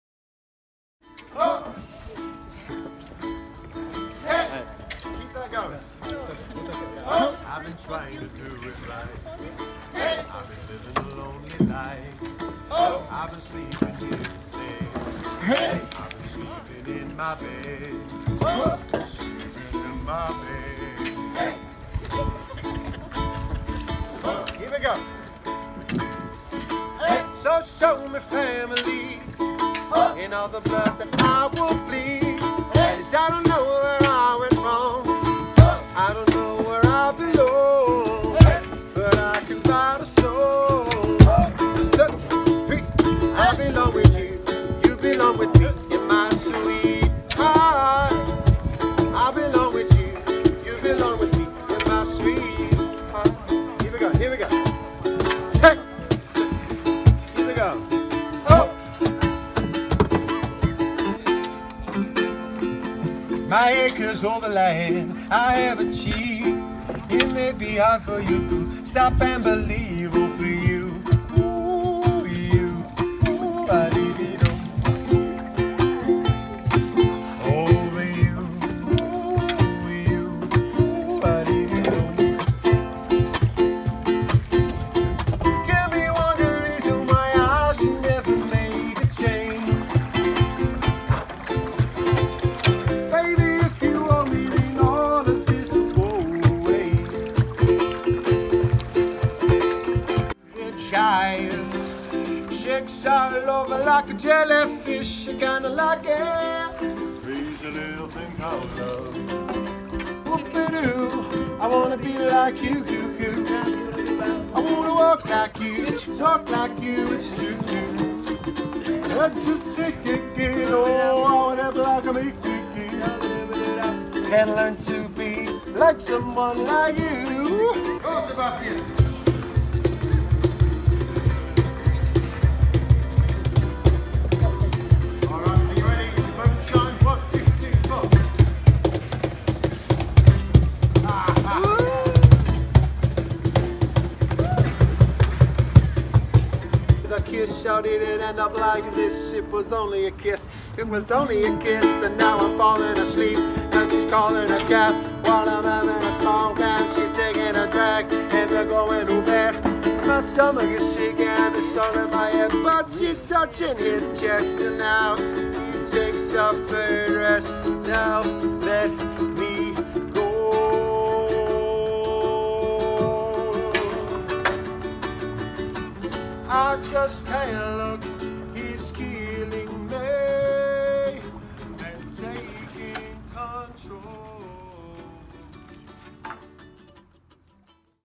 Music on the River with Ukulele Simon | Traditional Punting Company
We had a lovely time on the River Cam with Ukulele Simon.

Traditional Punting Cambridge
Punting Landing Stage Thompsons Lane, Cambridge CB5 8AQ, United Kingdom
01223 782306